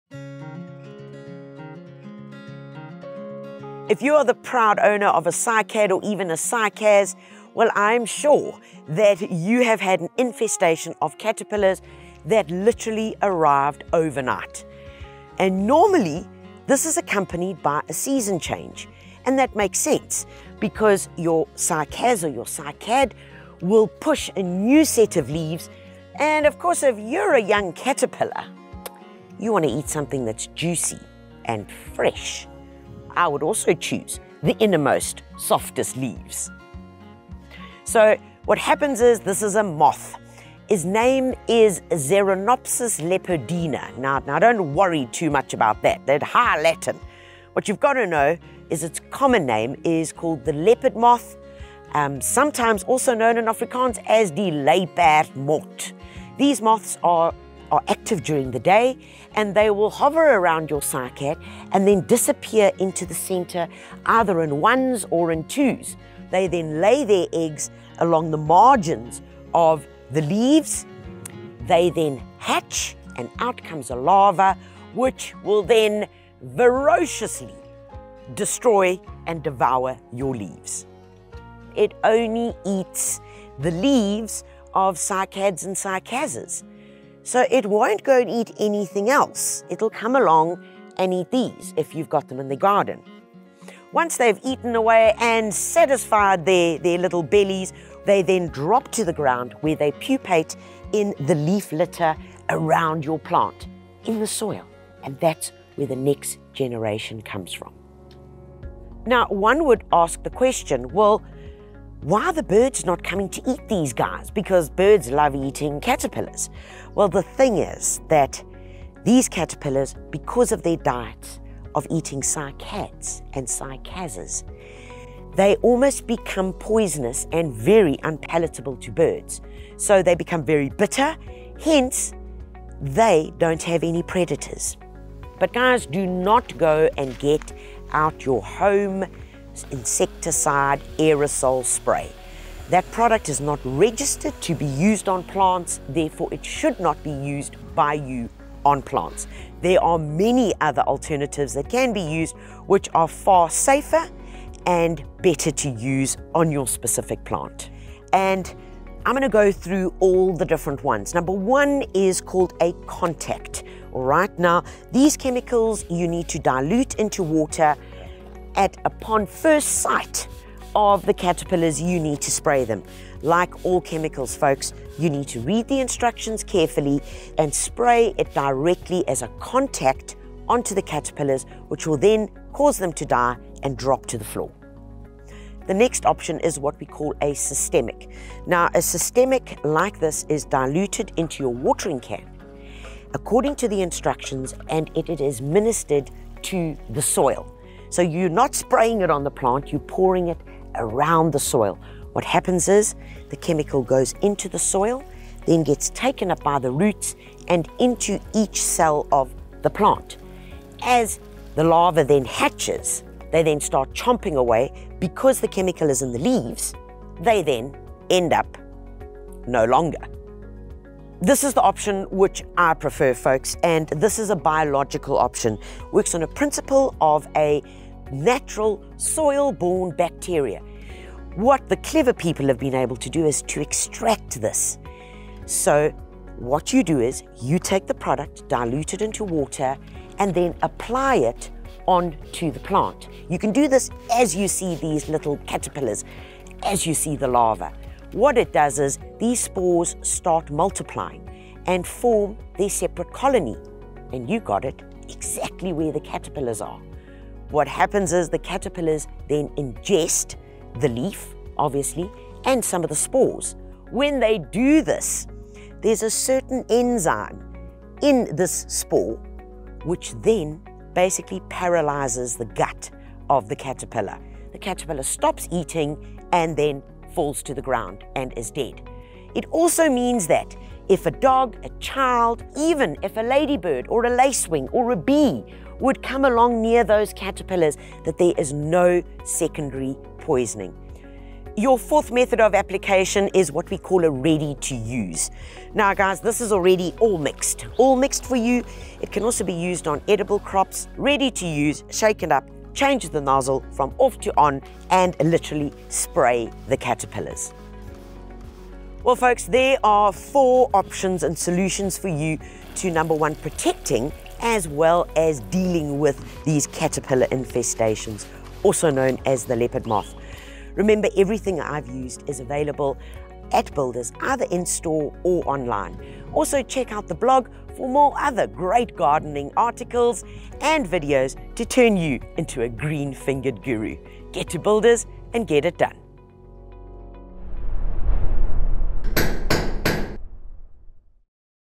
How To Handle Caterpillars On Your Cycads
Cycads are often attacked by caterpillars, especially during the change of season when they sprout new leaves. Their diet makes them unpalatable to birds so they are free to grow into the leopard magpie moth and the process starts again. Watch Tanya Visser as she discusses four safe options with different applications that you can use to get rid of there caterpillars on your cycad plant.